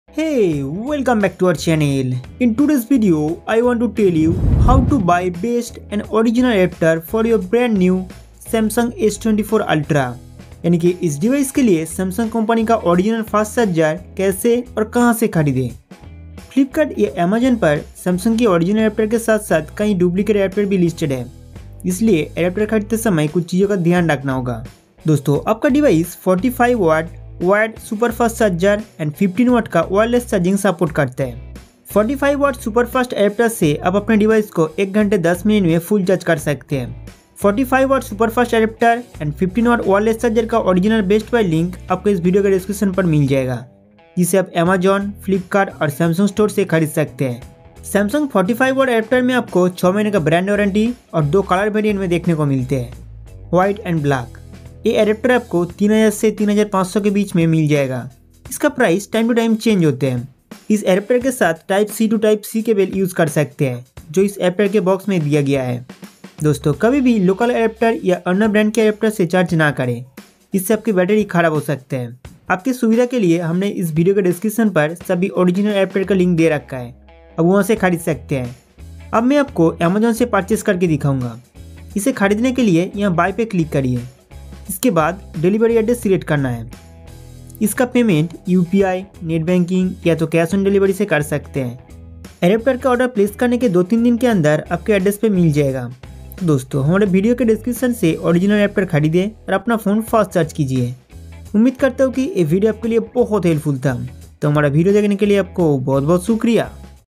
Best Adapter for Samsung Galaxy S24 Ultra, Does it Support Wireless Charging?
How to Order Original Adapter for Samsung Galaxy S24 Ultra | Samsung Galaxy S24 Ultra has Support 45W Wired Super Fast Charger & 15W Wireless Charger, 45w Adapter Buy Link on Amazon | Samsung Galaxy S24 Ultra 5G Charger Buy Link on Flipkart

*Samsung S24 Ultra* Charging Speed Test: 0% to 100%

45W Adapter ⚡ 1 Hour 6m

25W Adapter⚡ 1 Hour 17m

🛒 SAMSUNG Original 25W Adapter (Buying Link)

🛒 SAMSUNG Original 45W Adapter (Buying Link)